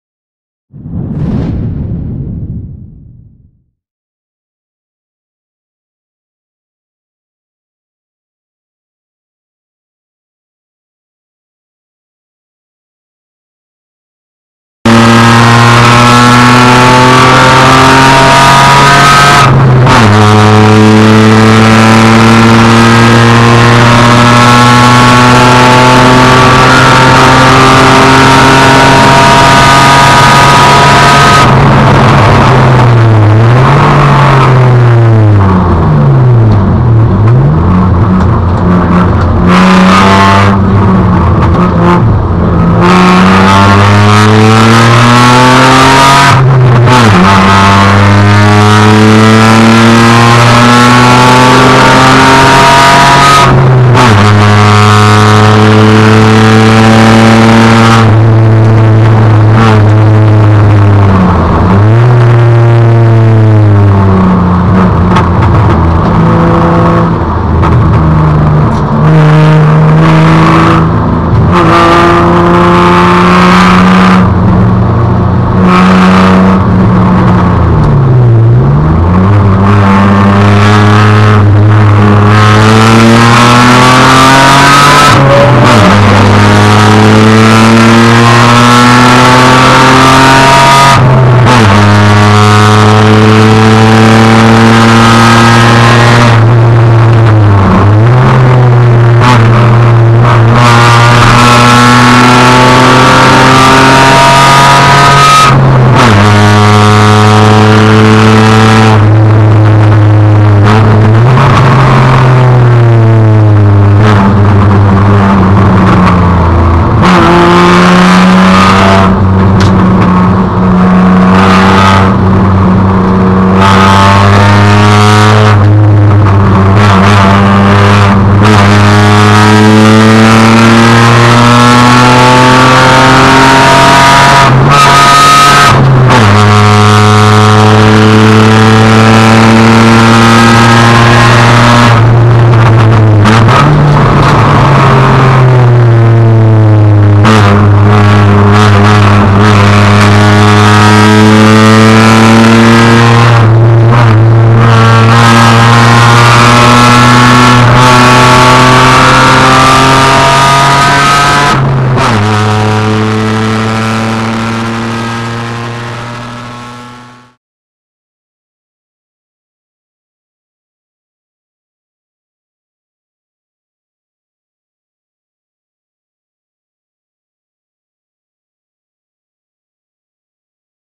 Amatérský okruhový závod Autodrom Most
Amatérský závod Le Most na okruhu Autodrom Most, který si může zkusit každý majitel řidičského průkazu. Velký nářez za volantem závodního speciálu BMW.